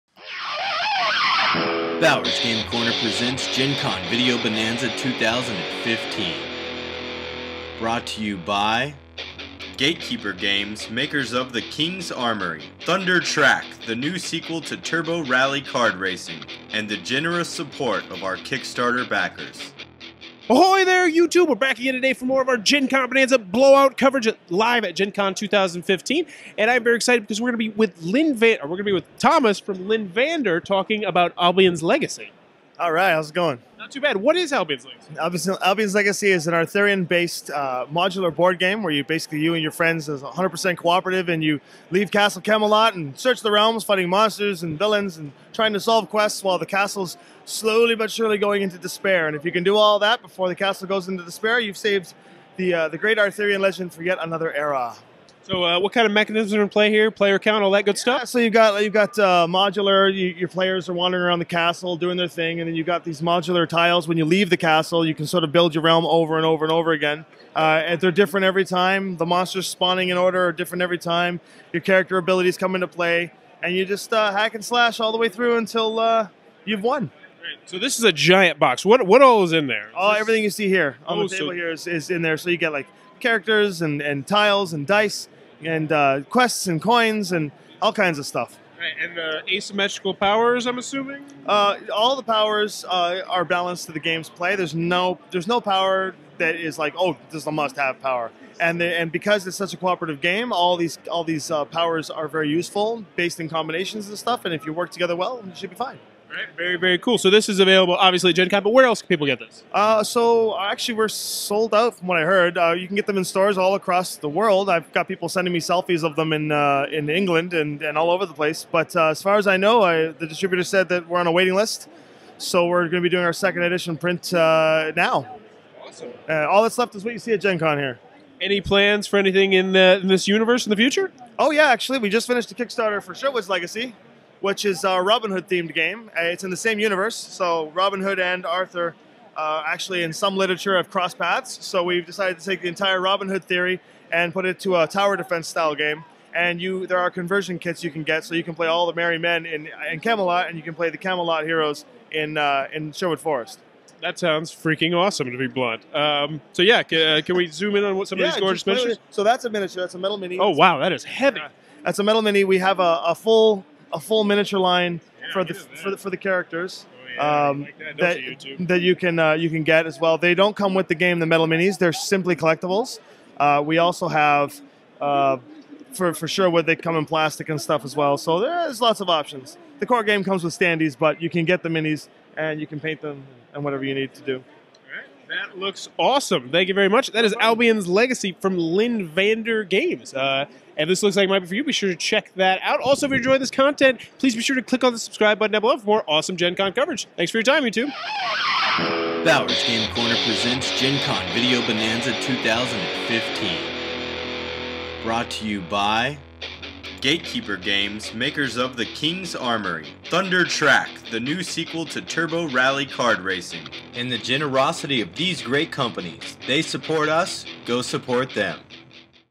Gencon Bonanza 2015: Albion's Legacy Interview
I talk with Lynnvander Productions About their upcoming game Albion's Legacy